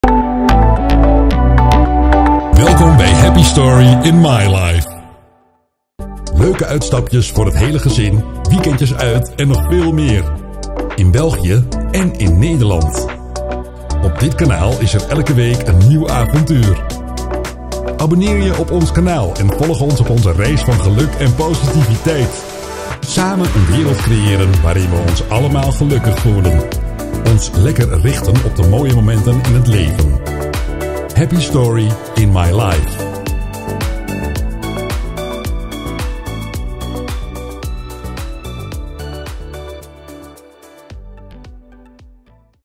#80 Introductie Trailer -The happy story in My life 2024
Welkom bij Happy Story In My Life! Op dit kanaal delen we positieve verhalen en ervaringen om je te inspireren en te laten zien dat het leven vol mooie momenten zit.

We gaan jullie mee nemen op heel leuke uitstappen met het gezin, weekendjes en nog veel meer.
Dat kan zijn naar leuke plaatsen in  België of Nederland die de moeite waard zijn om te bezoeken.

Je zal dan ook via ons kanaal ons kunnen volgen elke week met een nieuw avontuur.

Onze droom is om binnen dit en enkel jaren een bus camper te kopen, Maar gaan eerst eentje huren om te kijken of het wat is voor ons is, maar dat komt zeker en vast goed.

Samen kunnen we een wereld creëren waarin we ons allemaal gelukkig voelen en waar we ons kunnen richten op de mooie momenten in het leven.

Gerd en Tinne